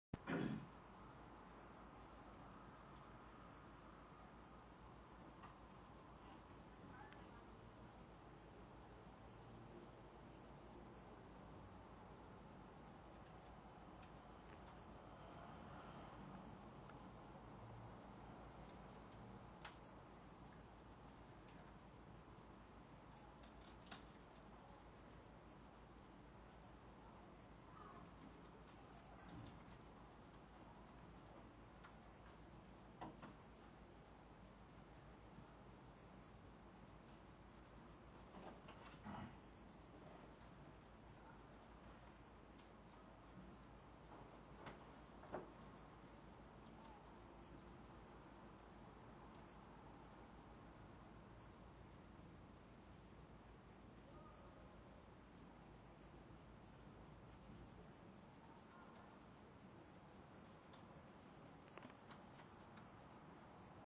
Kid Doing Karate In Front Of My Building
He doesn't give a shit. He does his karate where ever he wants. Punching air is much harder than punching people.